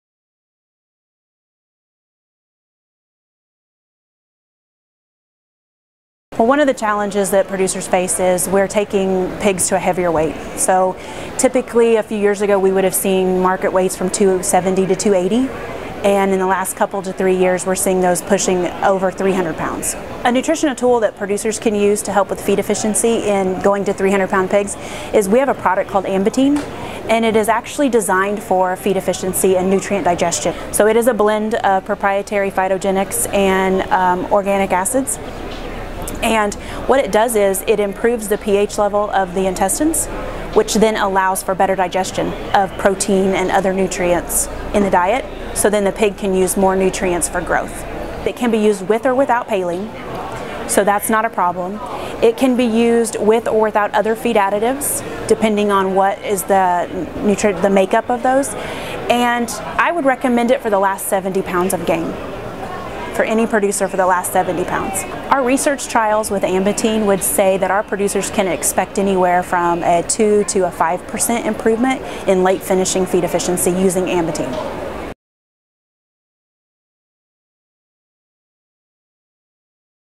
Ambitine™ for efficiency in late finishing
Over the last 10 to 15 years, the U.S. swine industry has seen a linear increase in pig market weights. A 285-pound finishing pig is common, and many plants are requiring 300-lb. finishing pigs. As pig weights have increased, so have the challenges in late-finishing, including feed inefficiency and heat stress.

Producers can profit with 300-lb. pigs if they can manage those challenges. Ambitine™ Feed Additive is a nutritional tool that has been shown to support 2% to 5% improvement in feed efficiency. Senior Nutritionist Stacie Crowder, swine technical solutions, explains why she recommends Ambitine™ in the last 70 pounds of finishing.